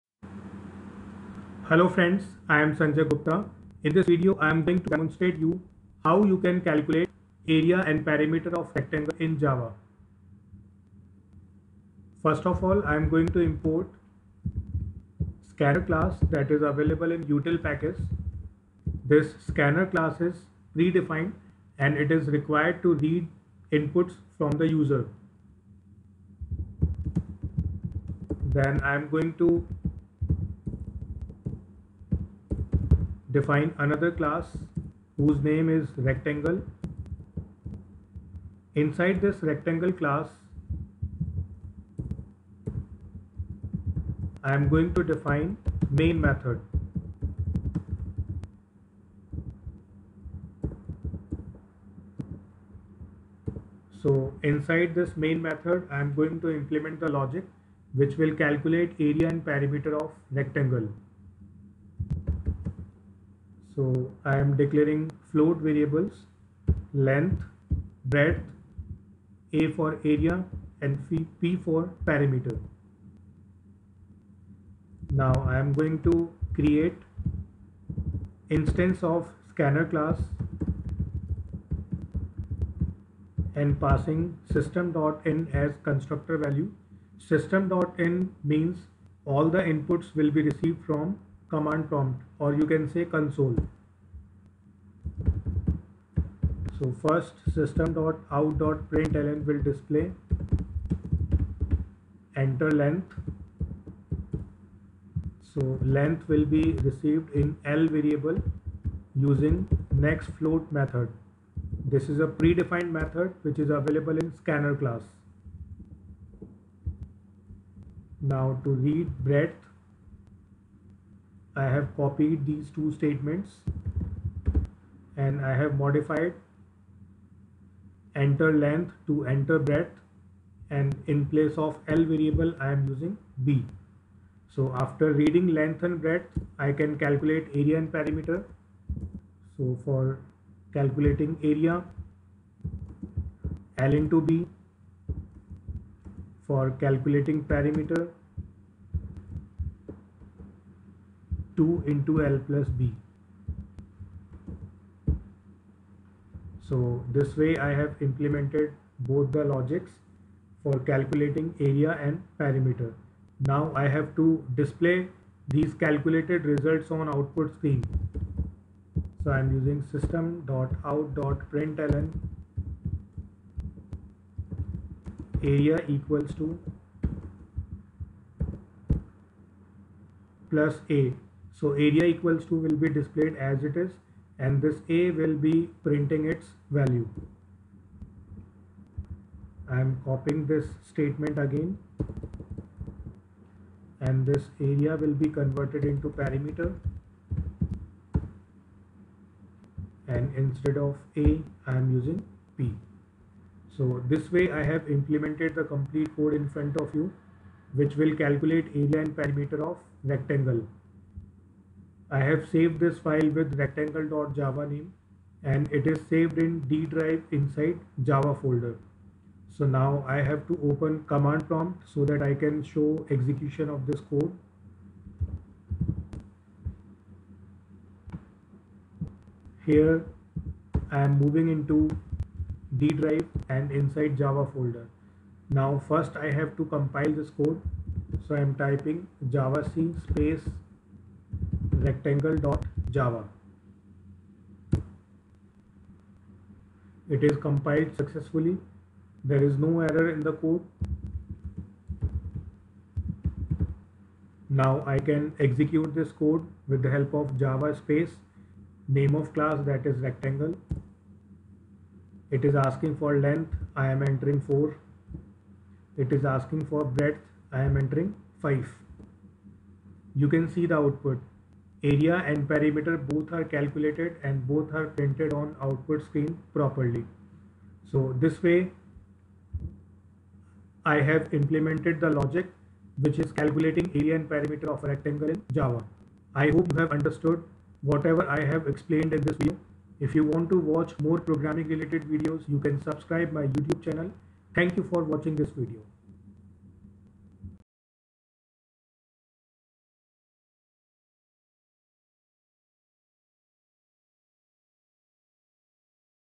07 Java Program | Calculate area and perimeter of rectangle | by Sanjay Gupta
How to calculate area and perimeter of rectangle in java programming language is demonstrated in this video tutorial.